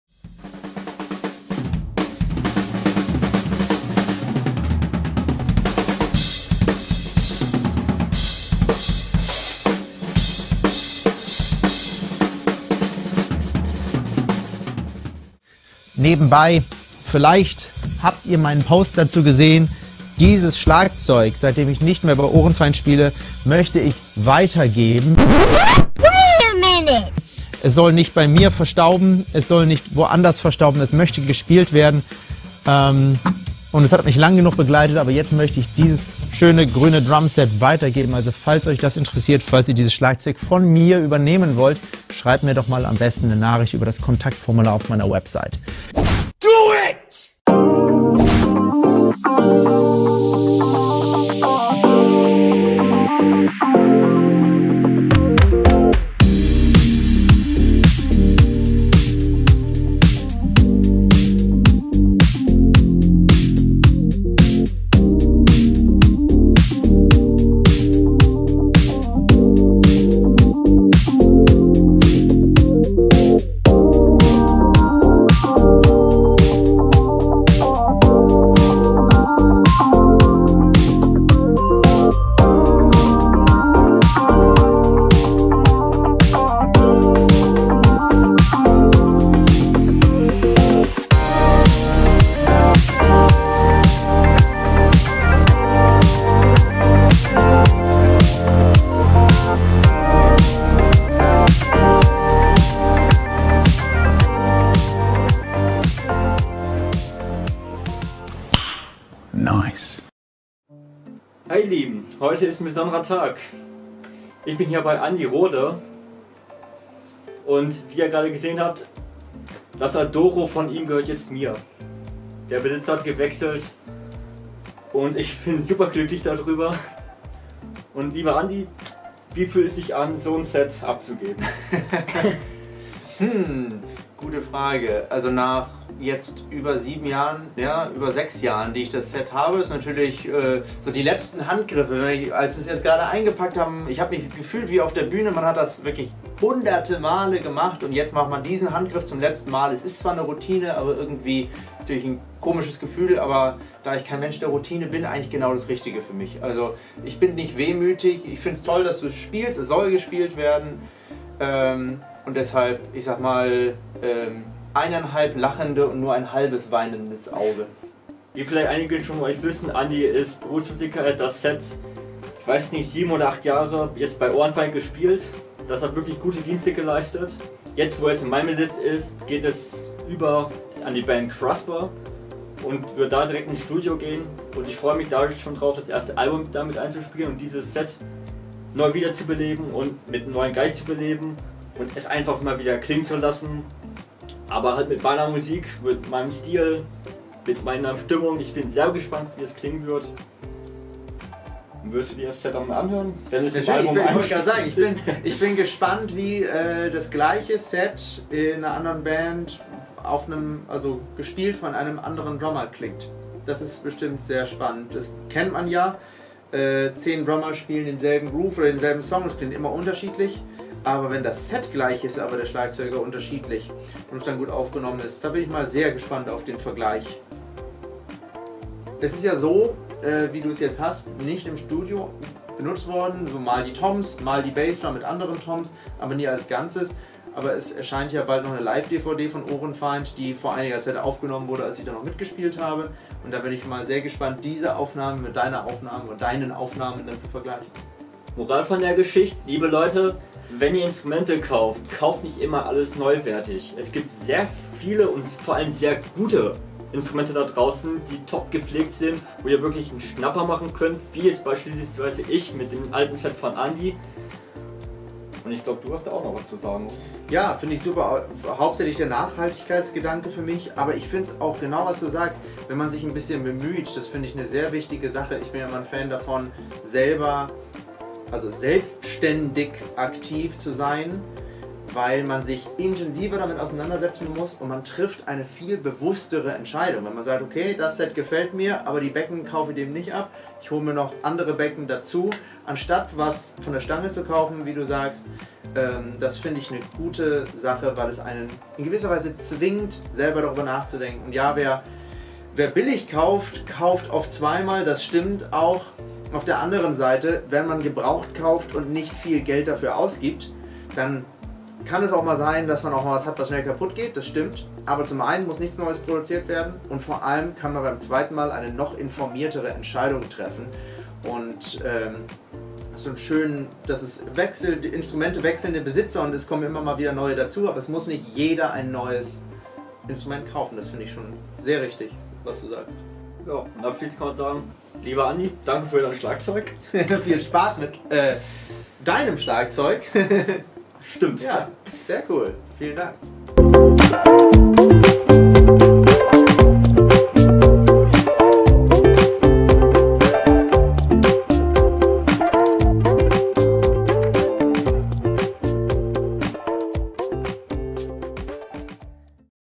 Darum war ich bei Andi Rohde
Am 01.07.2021 war besonderer Tag.

Ich habe Andi Rohde besucht und habe ihm sein Adoro Custom Drum angenommen.
Nach 7 Jahren bei Ohrenfeindt geht es jetzt an die Band Crossbar aus Hameln über.